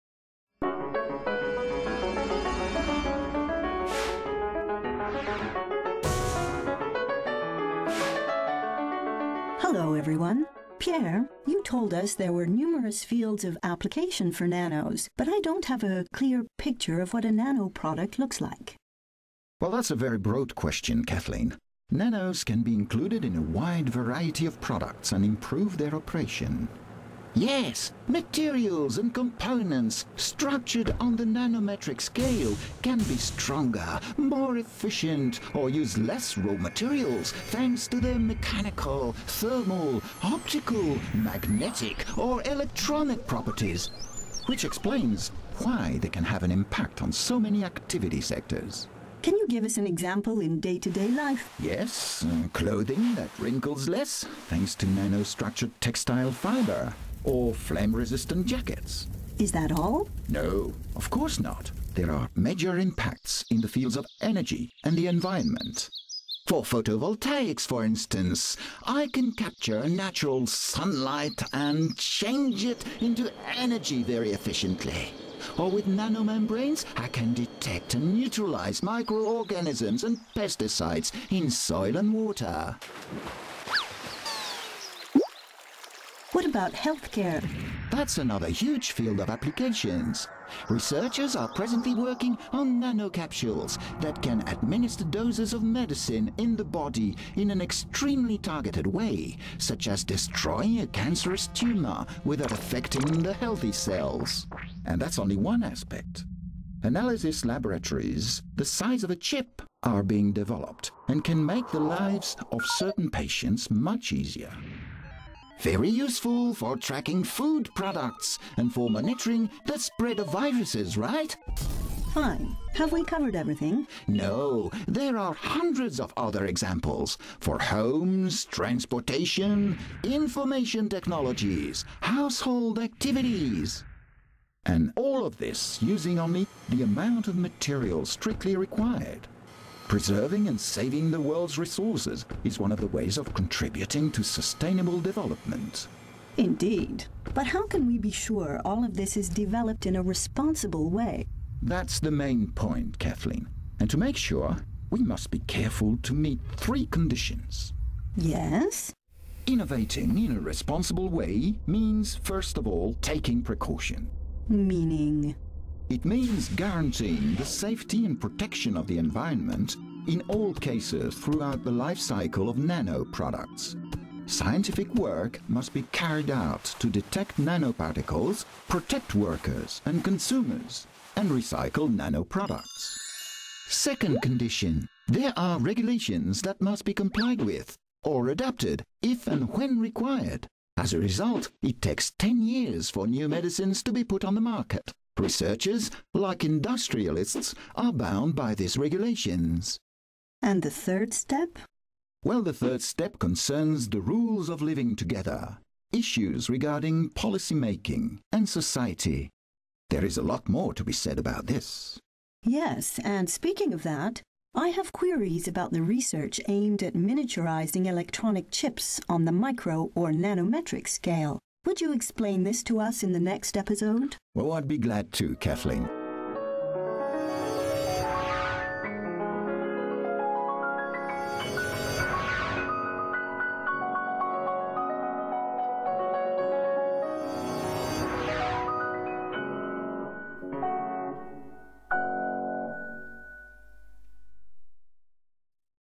What applications and products are they used in ?
Discover the many different applications for nanoparticles in a wide range of industries, including IT, energy, transportation, healthcare, and environnemental protection. Learn the three pillars of responsible innovation : take appropriate precautions, behave ethically, and ensure compliance with appropriate regulations.